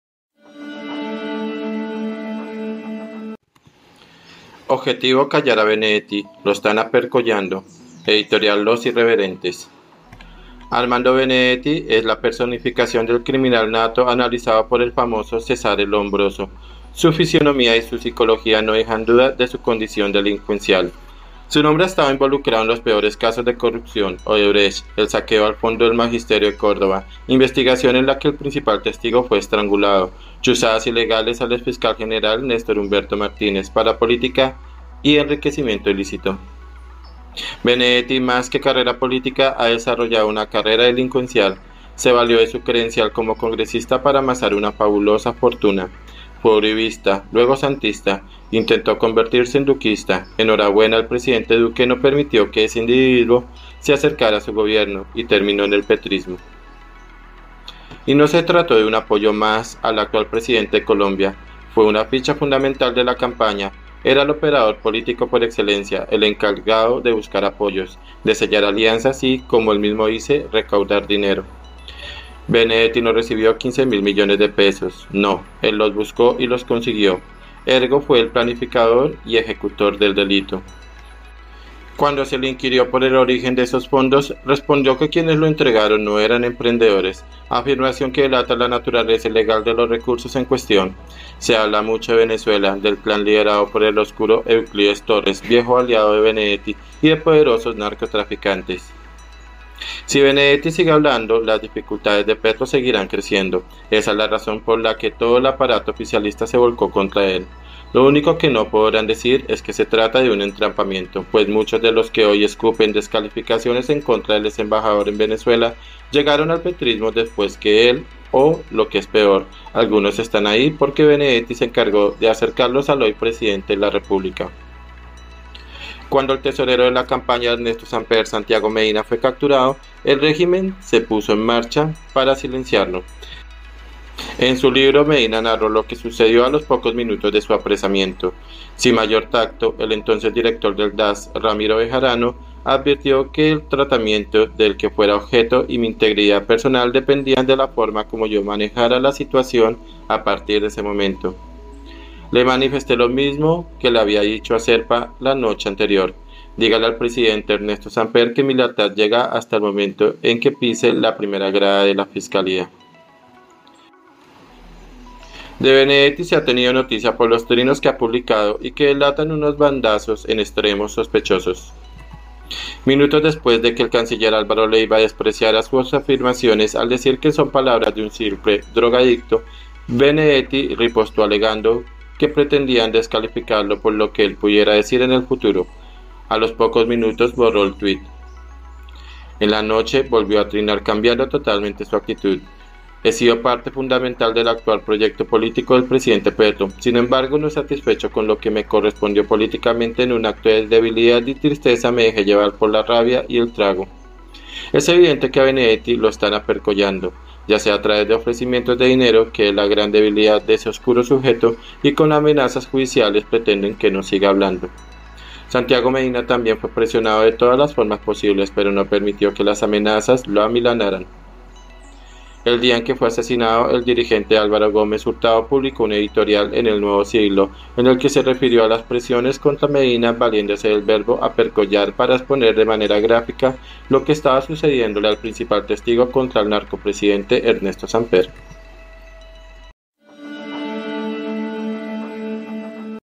OBJETIVO: CALLAR A BENEDETTI || LO ESTÁN APERCOLLANDO 🔴 Los Irreverentes
Su nombre ha estado involucrado en los peores casos de corrupción: Odebrecht, el saqueo al fondo del magisterio de Córdoba, investigación en la que el principal testigo fue estrangulado, chuzadas ilegales al exfiscal general Néstor Humberto Martínez, parapolítica y enriquecimiento ilícito.